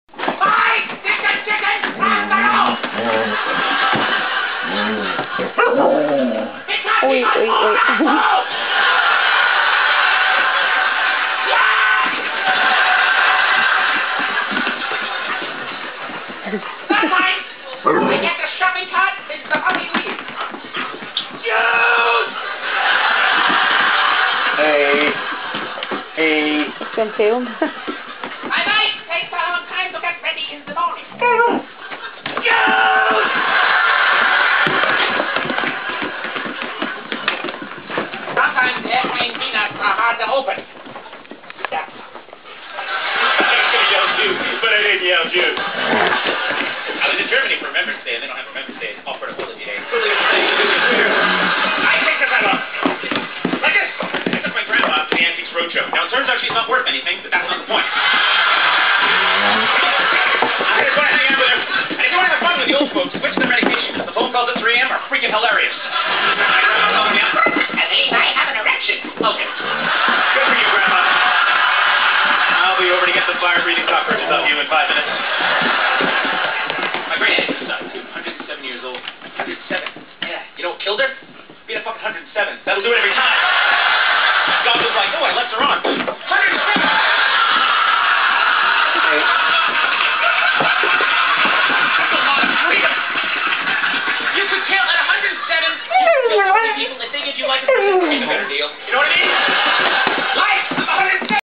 Copper & Blossom
Copper and Blossom playing Foxy and the Chihuahuas in background and nala is crated Sasha is outside with the sheepy :)